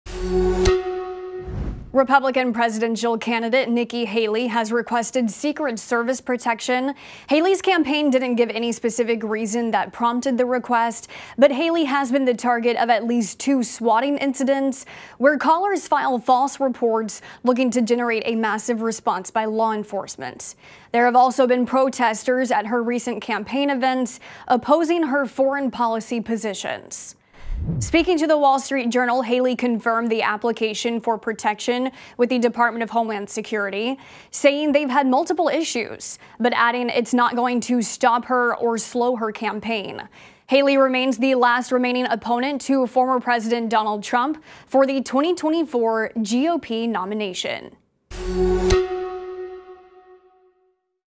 Nikki Haley applies for Secret Service protection
Republican presidential candidate Nikki Haley has requested Secret Service protection. Haley’s campaign did not provide any specific threats that prompted the request. Still, the former ambassador to the United Nations has been the target of at least two “swatting” incidents. According to a Reuters report, one incident on Dec. 30, 2023, and one on Jan. 1, 2024 where callers filed false reports looking to generate a significant response by law enforcement.